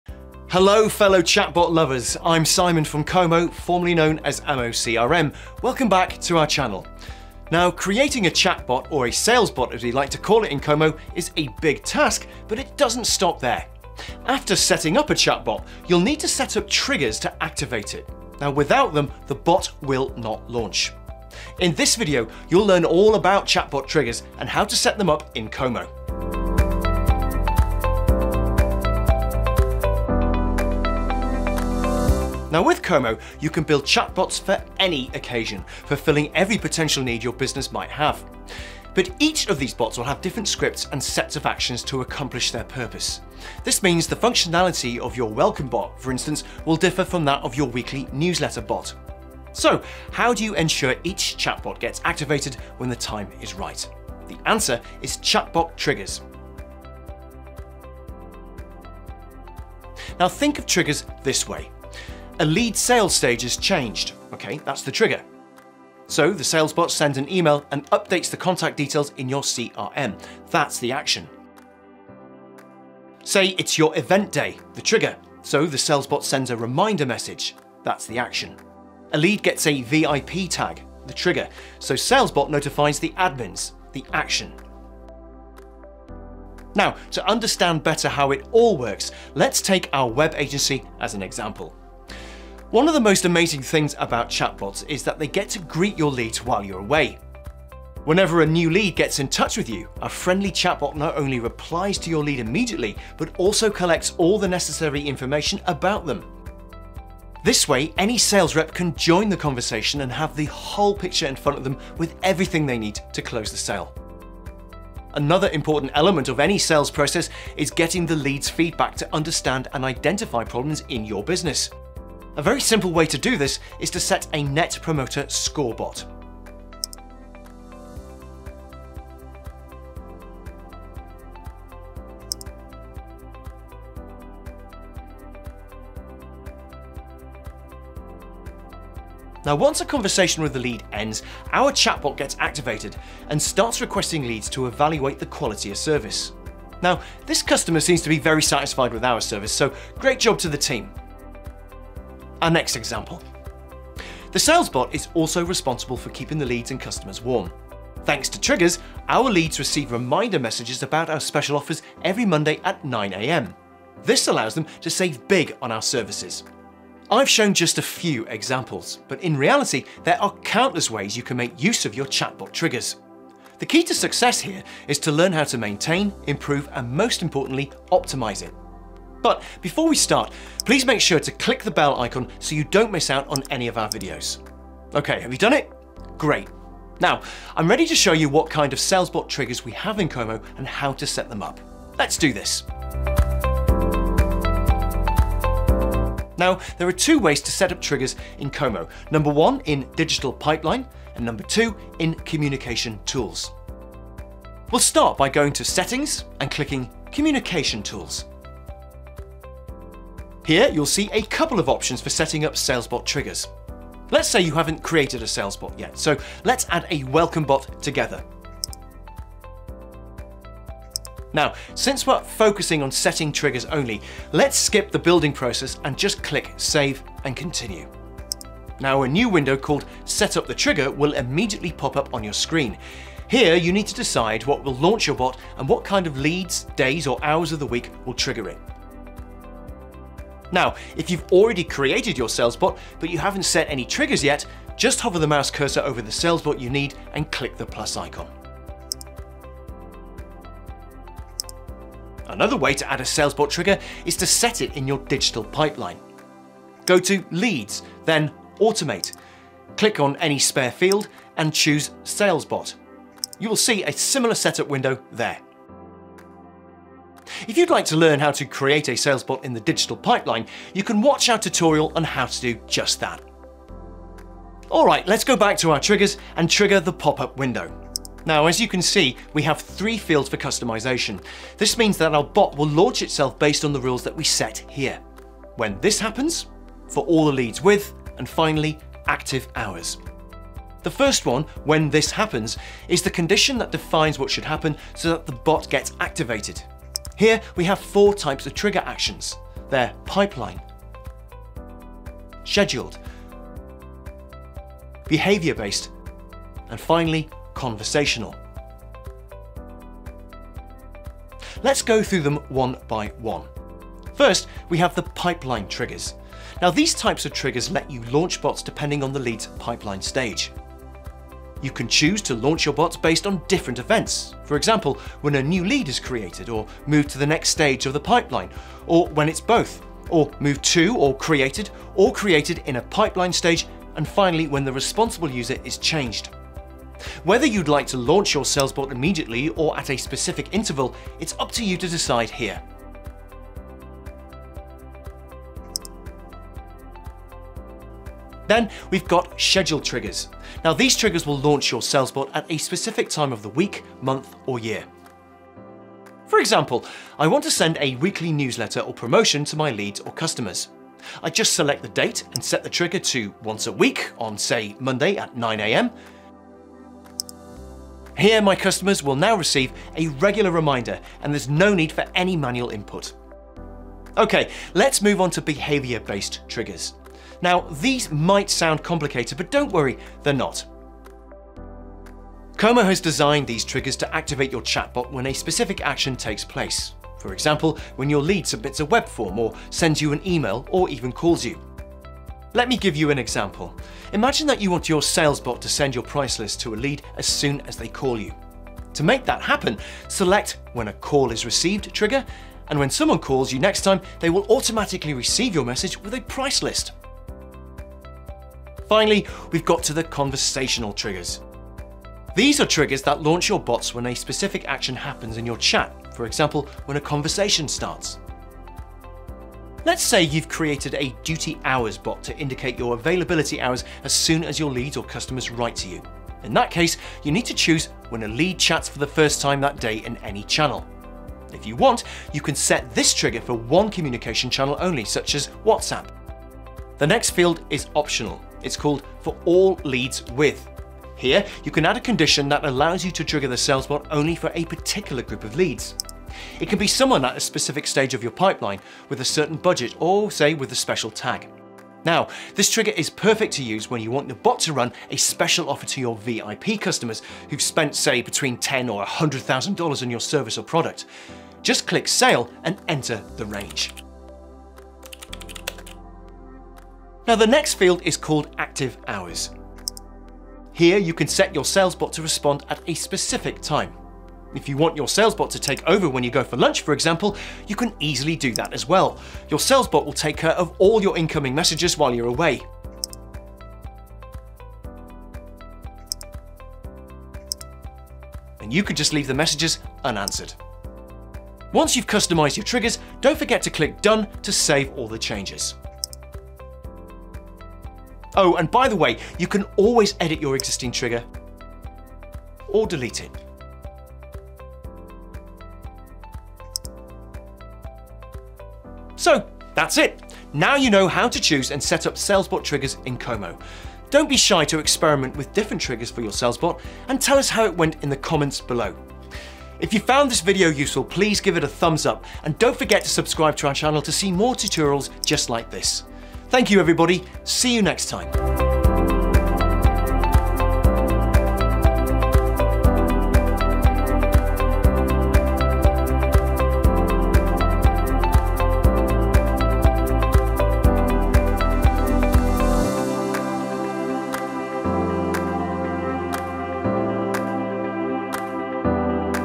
How to set chatbot triggers l Kommo Salesbot
Building a chatbot is only half the task. Customizing it with the triggers is when it can actually bring the most benefit to your business. In this video, you will learn how to set chatbot triggers in Kommo (ex amoCRM) and how it can enhance customer experience.🤓

00:00 Intro
00:30 Why do I need a Trigger for a Chatbot?
00:59 What do Salesbot triggers do?
01:35 Example 1 - While you're away
01:58 Example 2 - Once a conversation ends
02:32 Example 3 - When Schedule time arrives
03:15 How to set up triggers in Kommo
04:49 Set up the trigger
05:15 Pipeline triggers
05:47  Scheduled triggers
06:18 Behavior-base triggers
06:50 Conversatrional triggers